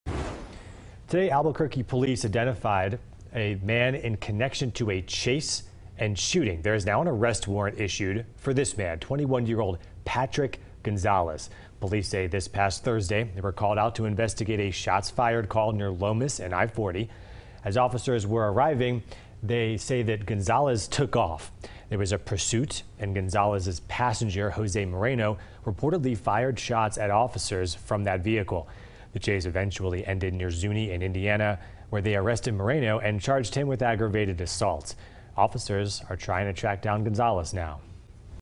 Arrest warrant issued for second suspect involved in police chase
Albuquerque police say they've identified the man in connection to a chase and shooting. An arrest warrant has been issued for 21-year-old Patrick Gonzales.